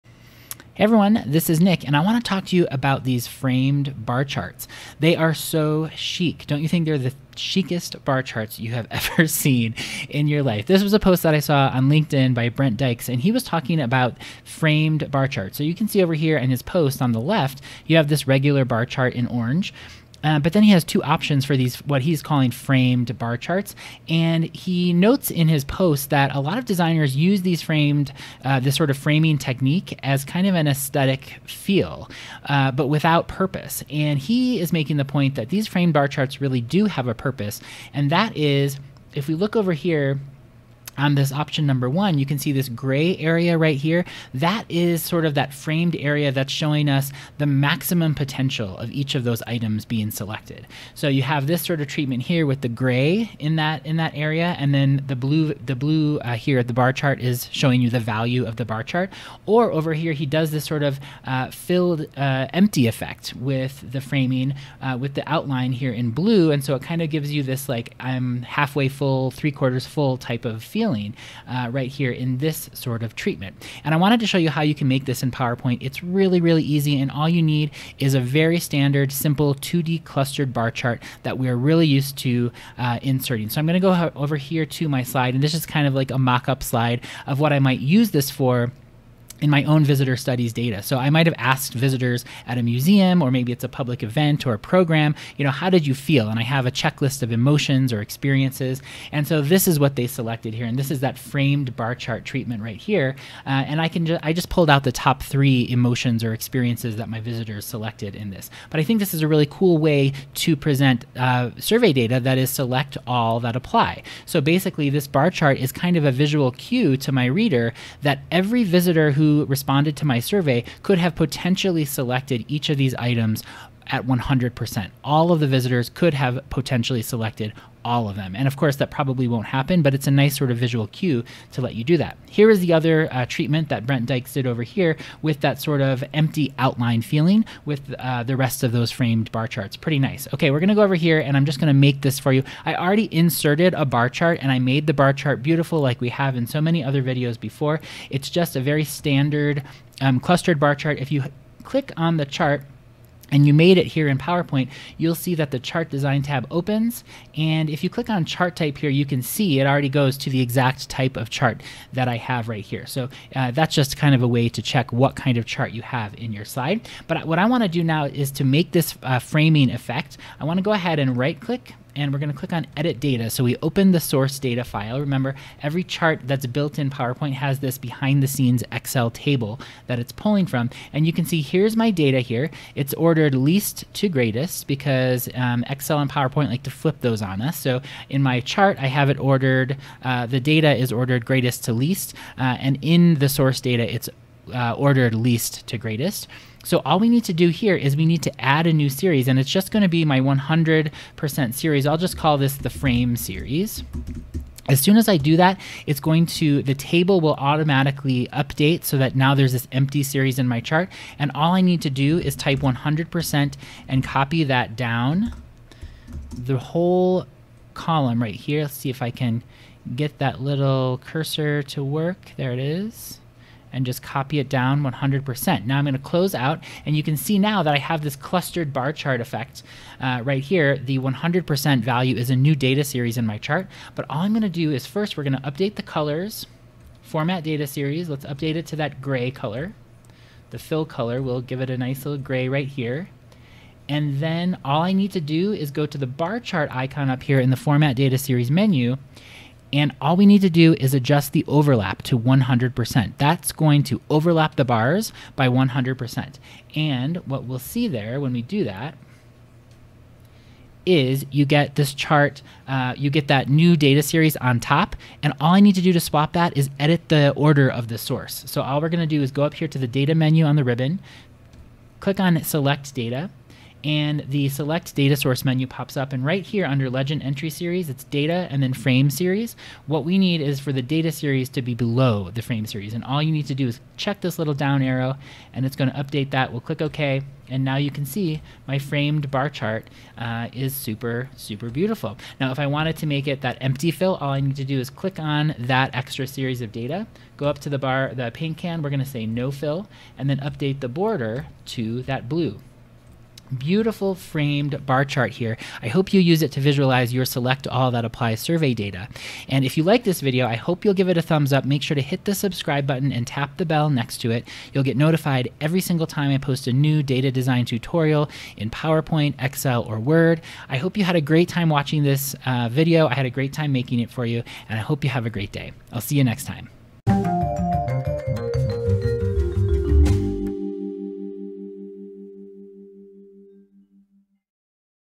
How to Make Stylish Bar Charts in PowerPoint 🔥 [PPT CHARTS 📊]
In this step by step tutorial you'll learn how to make a FRAMED bar chart. These are the chicest bar charts you've ever seen, Framed Bar Charts! Brent Dykes over on LinkedIn made a post about these and I hadn't heard of them called Framed Bars before, but makes total sense! I use these a lot to visualize visitor studies data from visitor experience or program evaluation surveys in museums, zoos, aquariums. It's great visual cue to let your reader know that all of your respondents COULD have selected each of the options in your survey question. Give Brent a follow YouTube or over on LinkedIn won't you!? He makes great data viz posts!